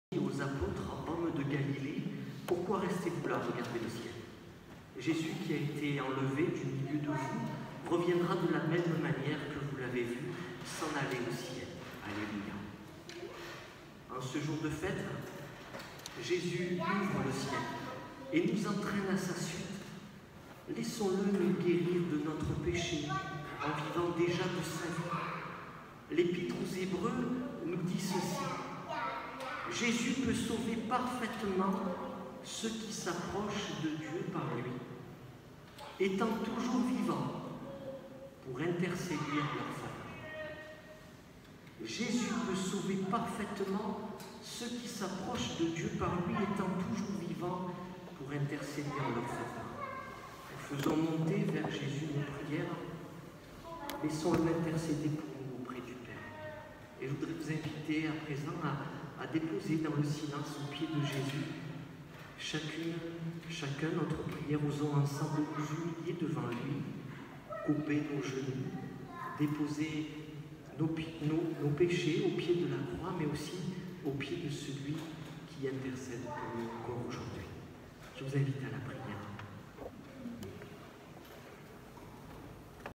Jesus monte au ciel devant les apotres ~ Ascension part 6 jeu 10 mai 18
Enregistré dans l'eglise rue de la Masse à Aix en Provence ~ Site officiel de l'église protestante unie